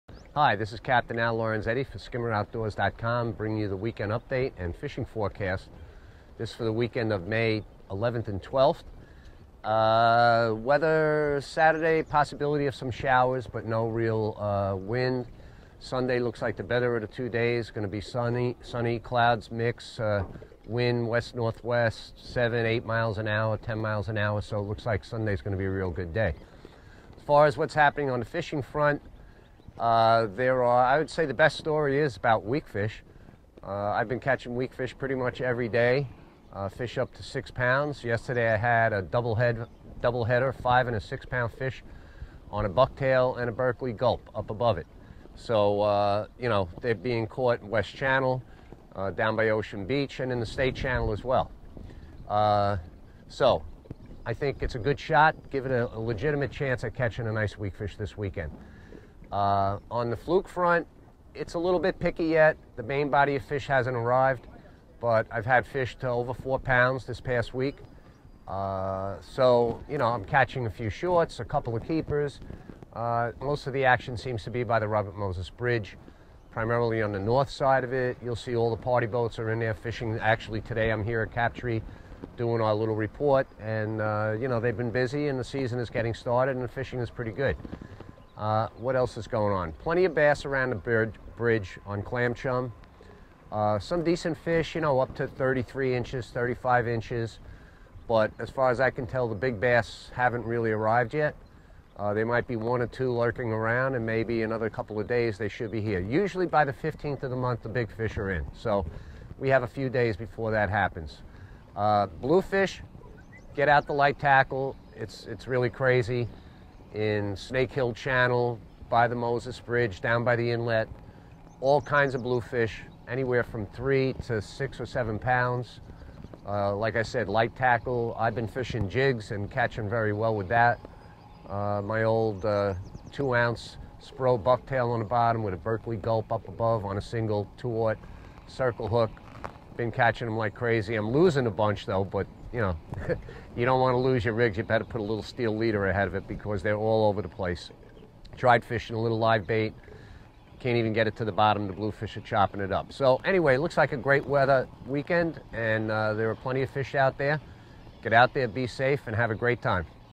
Fire Island Fishing Forecast for May 10 11 12
Captain Al Lorenzetti's weekly weekend fishing forecast for the Fire Island and Great South bay area.